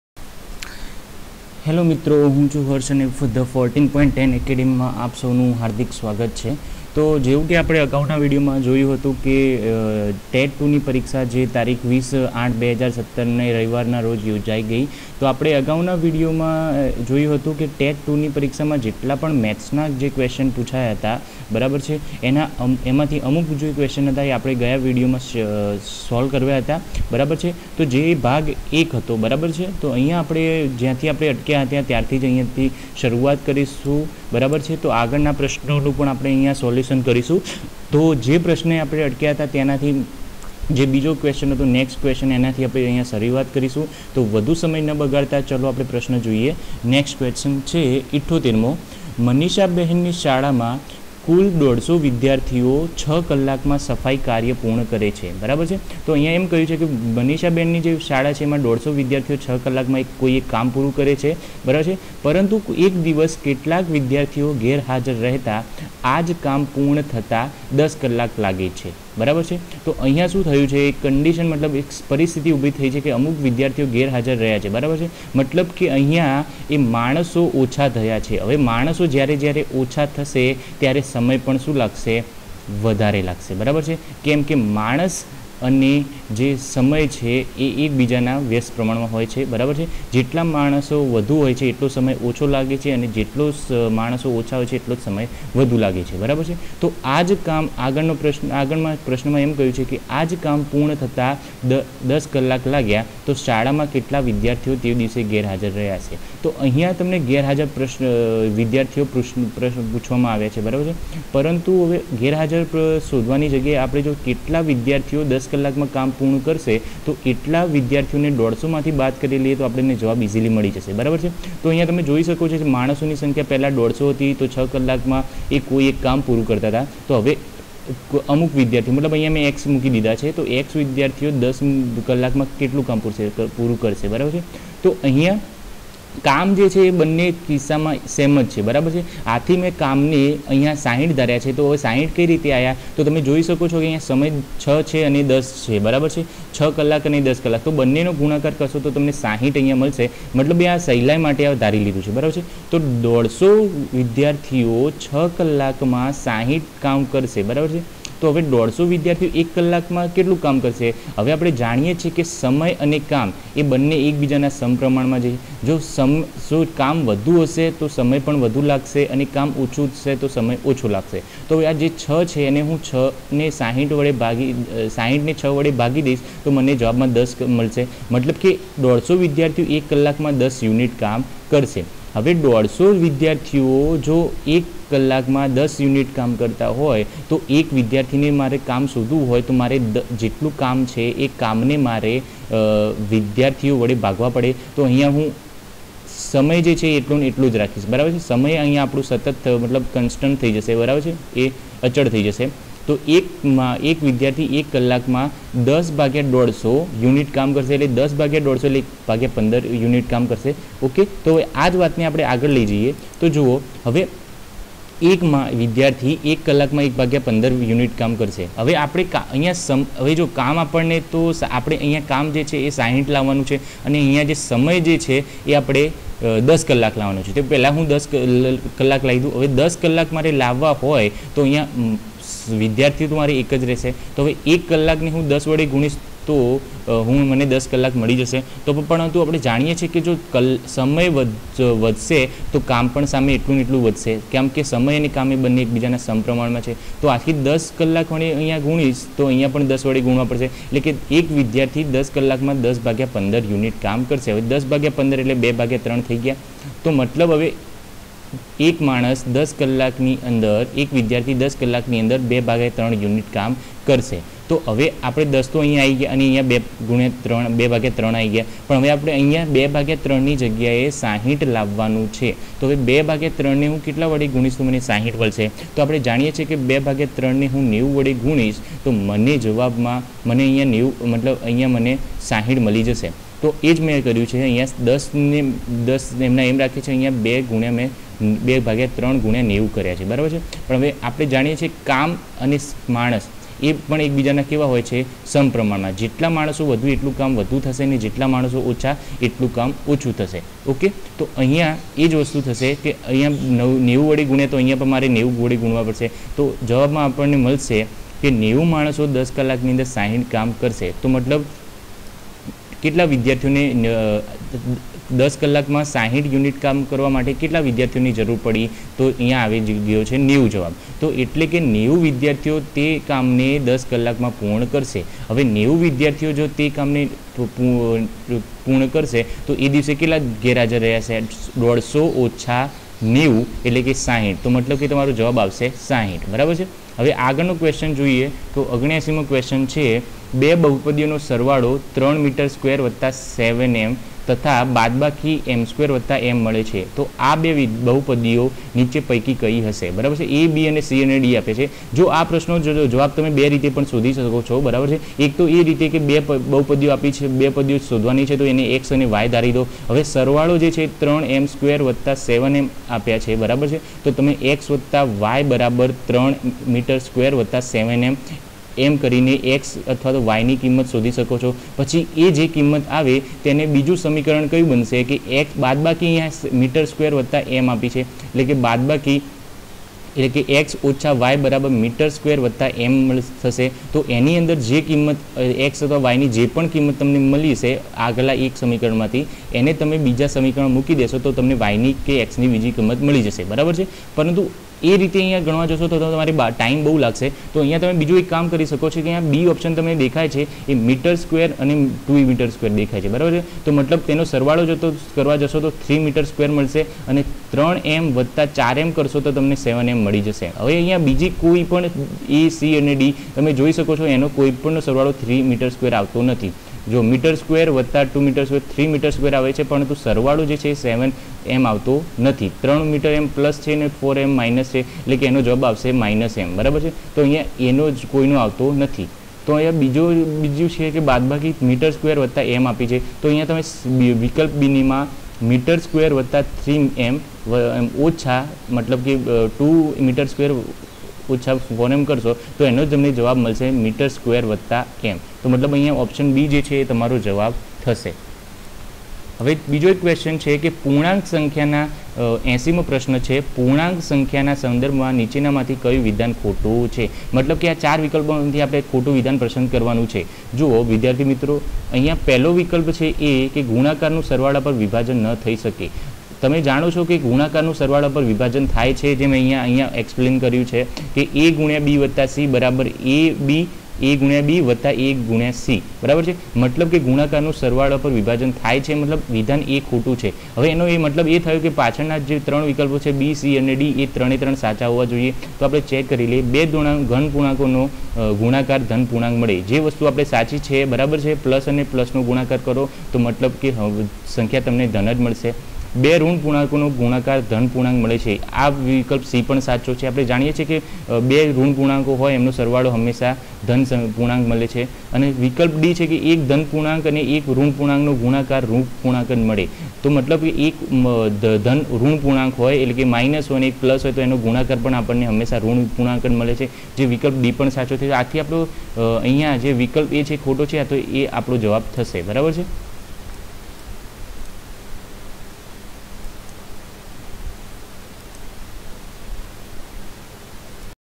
TET -2 EXAM 2017 MATH SOLUTION PART 2
હેલ્લો મિત્રો ,આજ વીડિઓનો વિષય છે ટેટ - ૨ ૨૦૧૭ પરીક્ષા નું ગણિત સોલ્યુસન ભાગ-૨. અગાઉના વીડિઓ માં આપણે ટેટ-૨ ની પરીક્ષામાં પુછાયેલ ગણિતના અમુક પ્રશ્નોને સોલ્વ કર્યા. જે ટેટ-૨ ૨૦૧૭ પરીક્ષા નું ગણિત સોલ્યુસન નો ભાગ-૧ હતો. આજના વીડિઓ માં આપણે છેલ્લા વીડિઓ માં જ્યાં અટક્યા હતા ત્યાં થી શરૂઆત કરીશું. દરેક પ્રશનું નું વધુ ઉંડાણથી સમજણ આપવાના અમારા પ્રયાસને લીધે અમે સમગ્ર ગણિત સોલ્યુસન ને ભાગ રૂપે વહેચ્યાં છે. ટૂંક સમયમાં બધા જ ભાગ મૂકીદેવામાં આવશે.